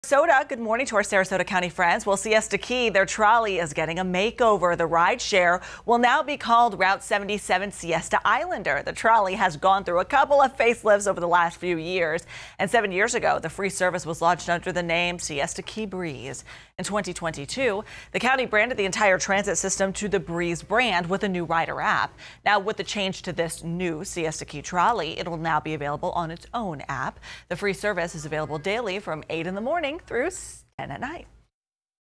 Changes are coming to Siesta Key trolley
The Sarasota trolley will now be available on a rider app for people looking to get around for free.